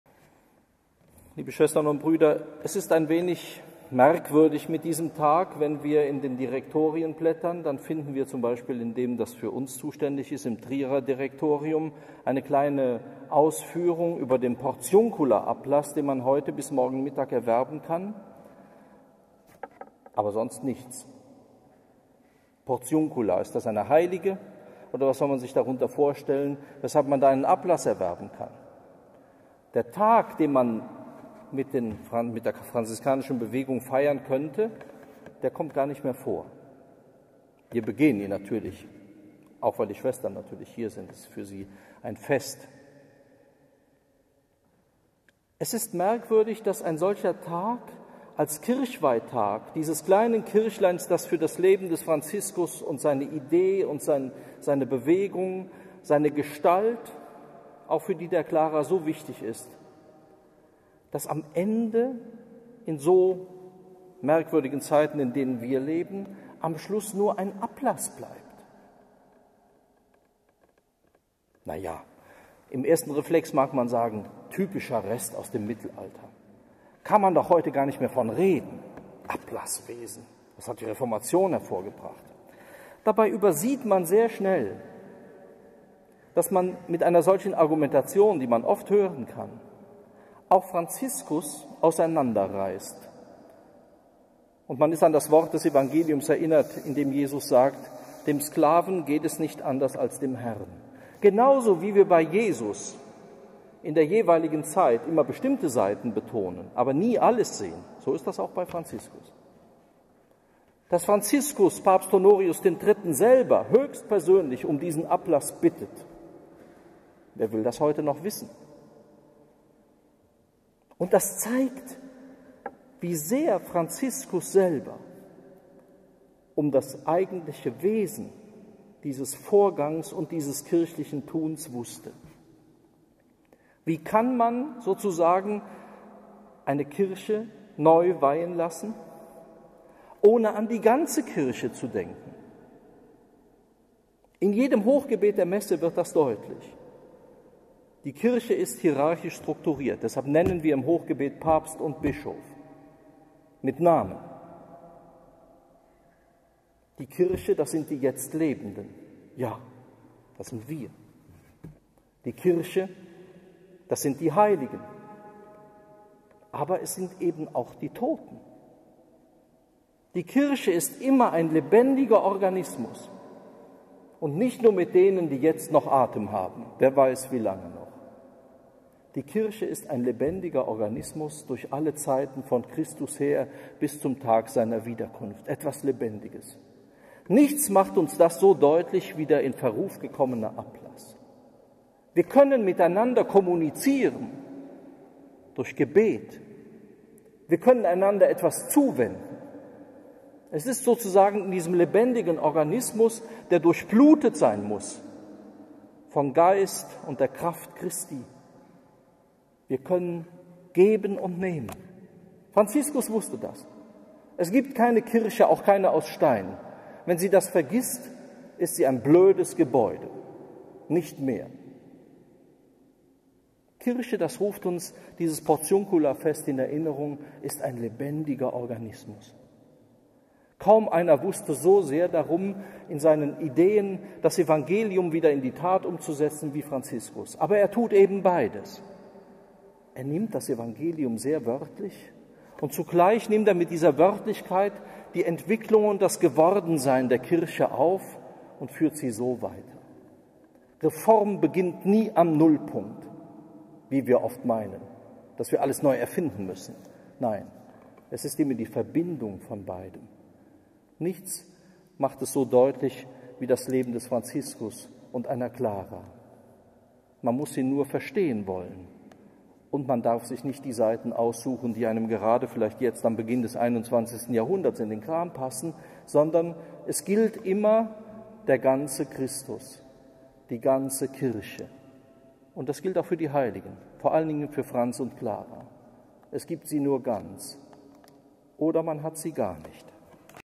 Im Direktorium nur noch eine Ausführung über den Ablass
Ausgerechnet darüber?
Hochschulpfarrer Dr. Kreier am Fest Portiunkula im Kloster am Rande der Stadt/ Saarbrücken.